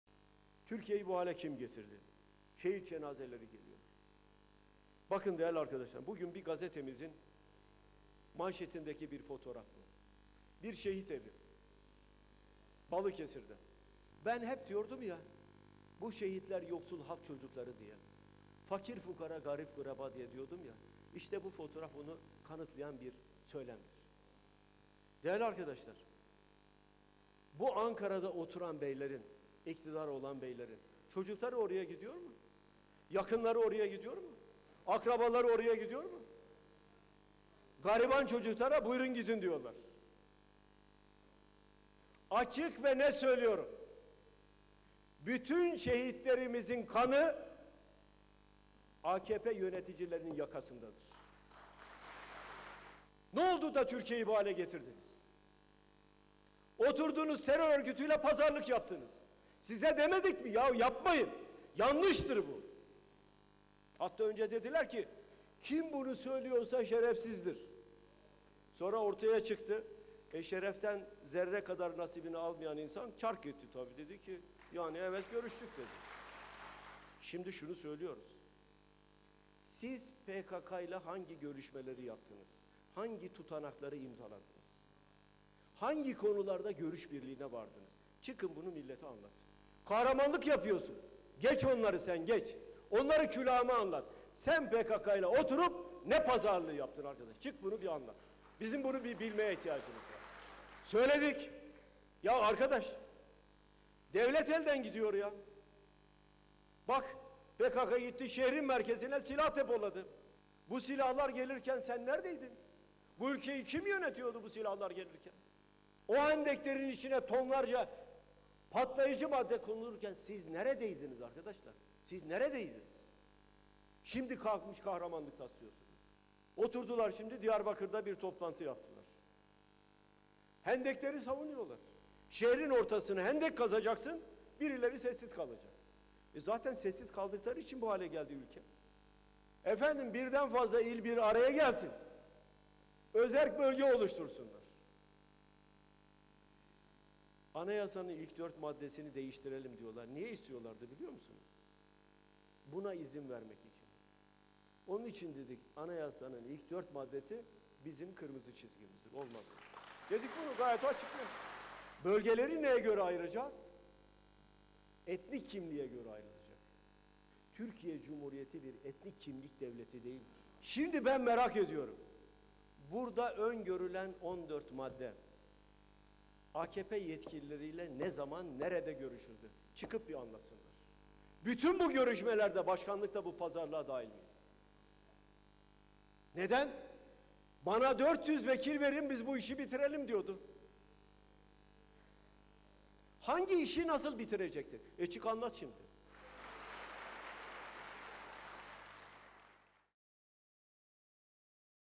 Türkiye Cumhuriyeti Etnik Kimlik Devleti Değildir!
Diyarbakır’da bir toplantı yaptılar. Efendim; "Birden fazla il bir araya gelsin, özerk bölge oluştursun, anayasanın ilk 4 maddesini değiştirelim’ diyorlar.  Onun için biz "Anayasanın ilk 4 maddesi bizim kırmızı çizgimizdir, olmaz" dedik. Şimdi ben merak ediyorum burada öngörülen 14 madde AKP yetkilileriyle nerede, ne zaman görüşüldü, çıkıp bir anlatsınlar.